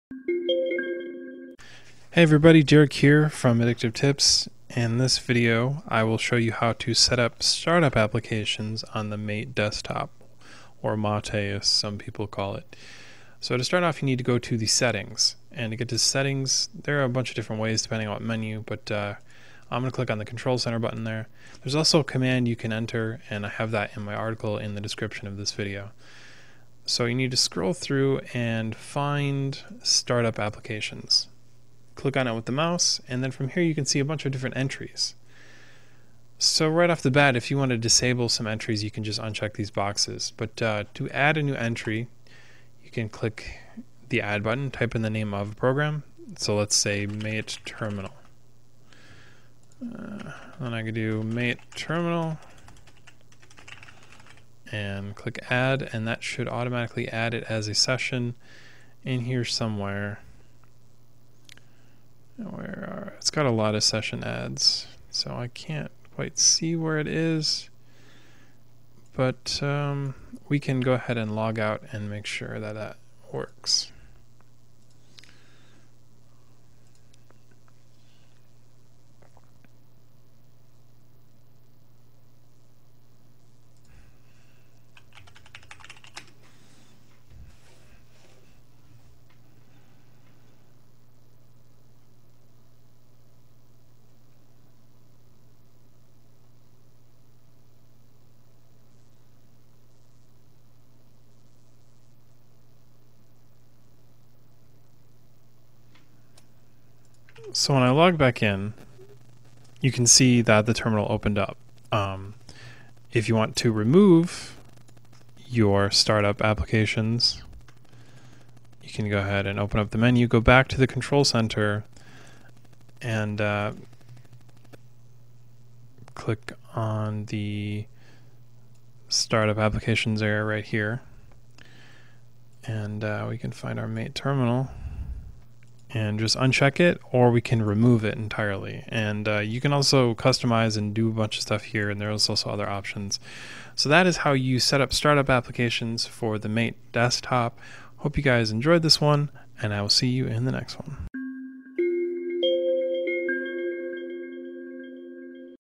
How to automatically start programs on Mate
The Mate desktop environment has a lot of settings that users can take advantage of for customization purposes. One of the best, most in-depth customization features is the autostart function, as it allows users to automatically start programs on Mate. They can decide how their Mate system loads up programs, scripts, and even services!